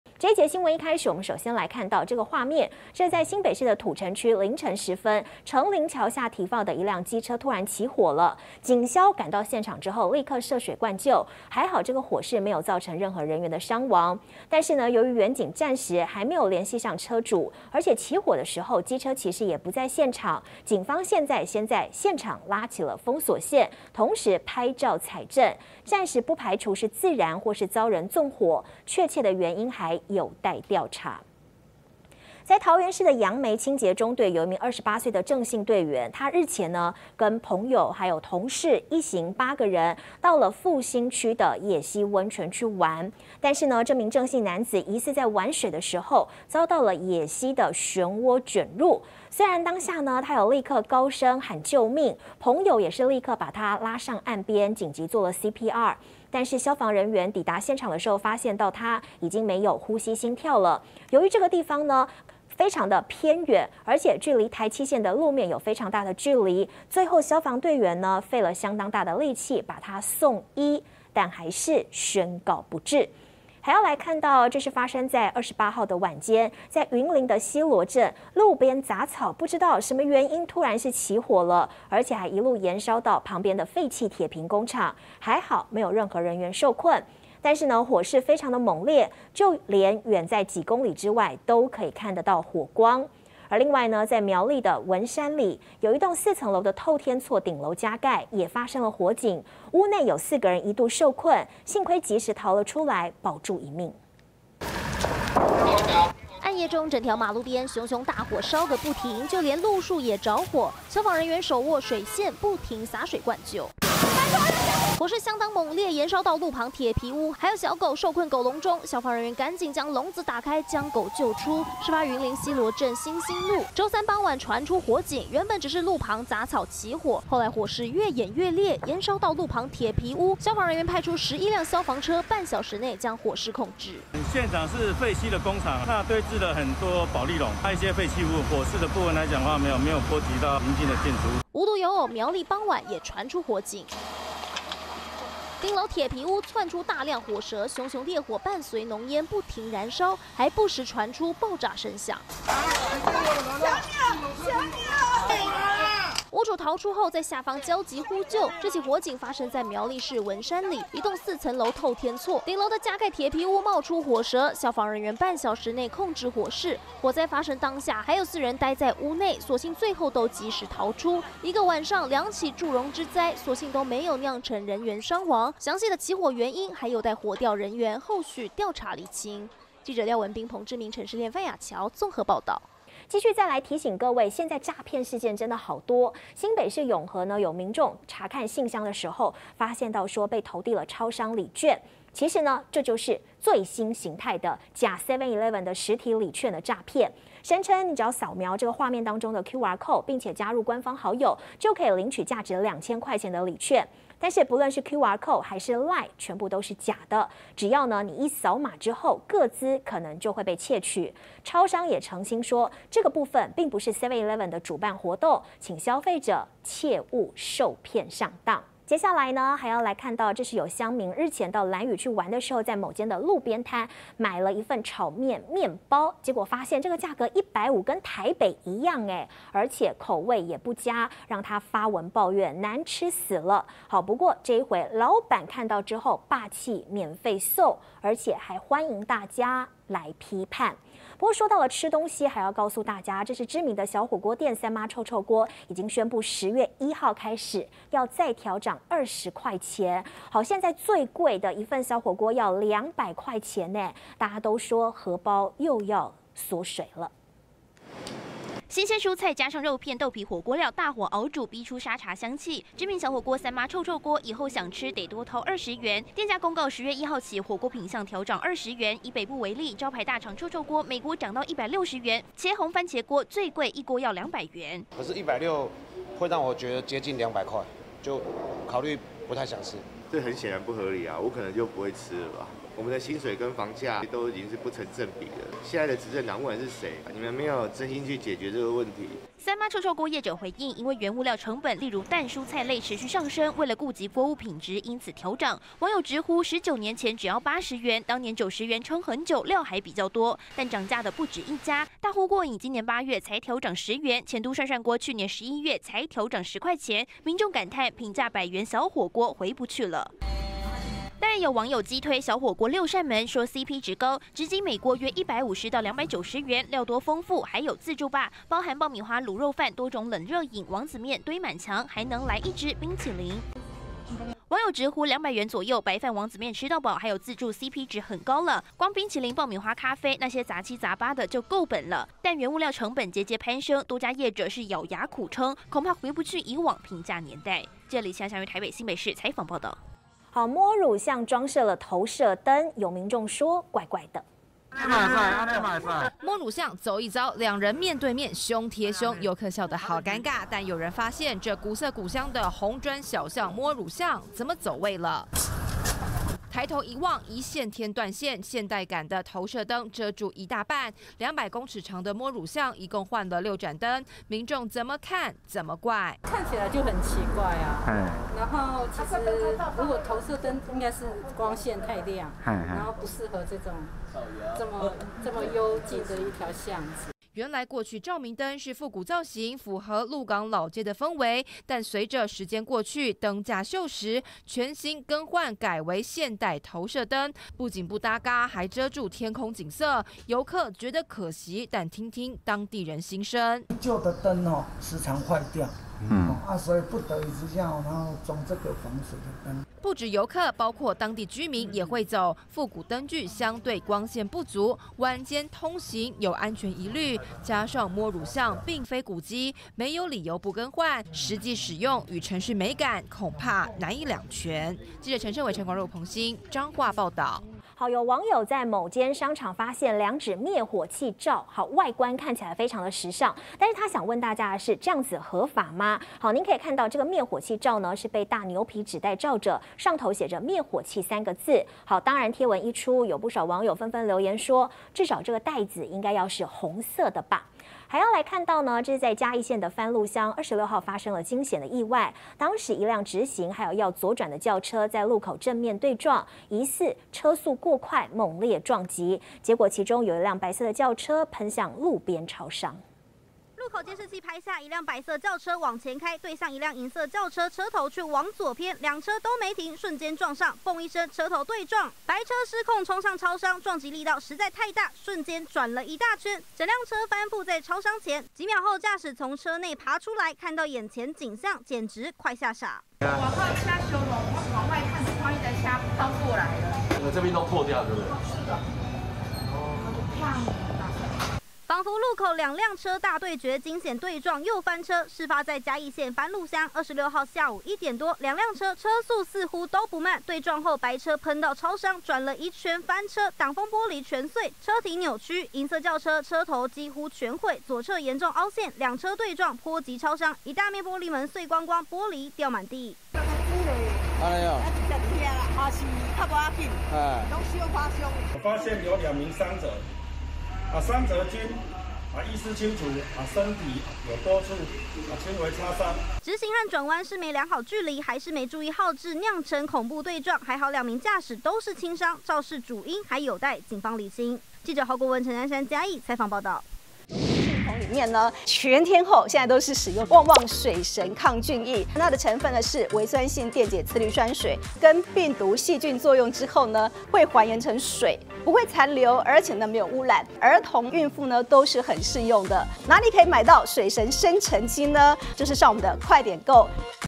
【每日必看】疑被捲進漩渦? 清潔隊員泡野溪溫泉慘溺斃｜土城橋下"機車起無名火" 警方不排除縱火可能 20220929
00:00 土城橋下"機車起無名火" 警方不排除縱火可能
00:32 疑被捲進漩渦 清潔隊員泡野溪溫泉慘溺斃
01:12 雜草延燒燬西螺鐵皮屋 苗栗頂加大火4人及時逃命
03:03 超商禮券掃描填資料送2千? 警政署:個資恐外洩
03:42 被嫌炒麵麵包"難吃" 老闆霸氣免費送:歡迎來批判
04:02 三媽漲價最貴2百元 平價火鍋剩"它"沒漲?
05:40 不搭嘎! 復古摸乳巷裝"投射燈" 一線天被剖半
06:57 商場滅火器"染文青風" 網酸:火災燒完就看得到
07:20 兩車對撞四輪朝天! 超商玻璃全碎 駕駛腿軟